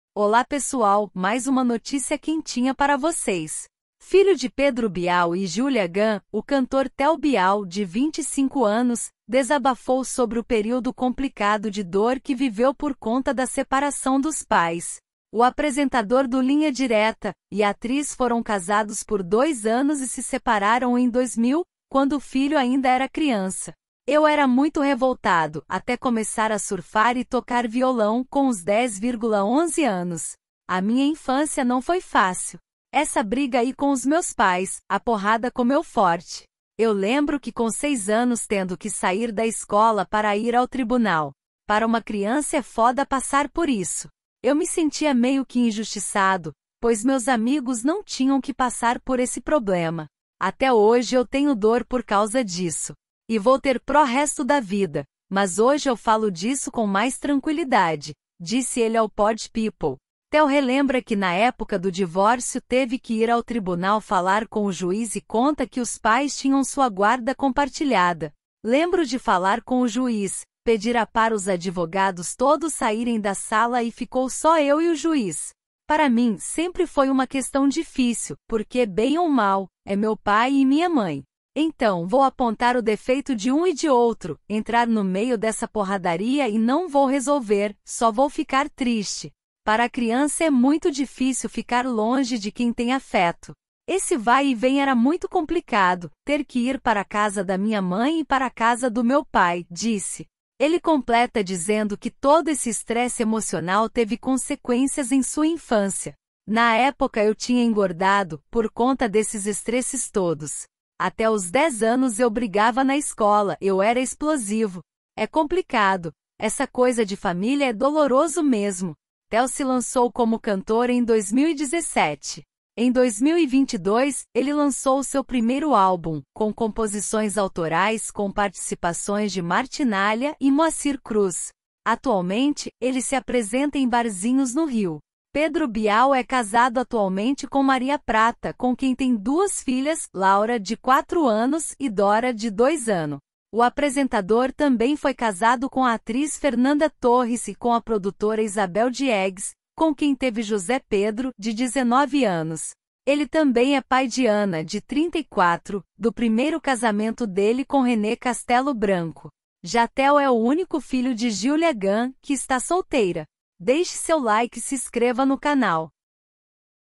Filho de Pedro Bial e Giulia Gam relembra conflitos na separação dos pais: 'Doloroso'
créditos : extra famosos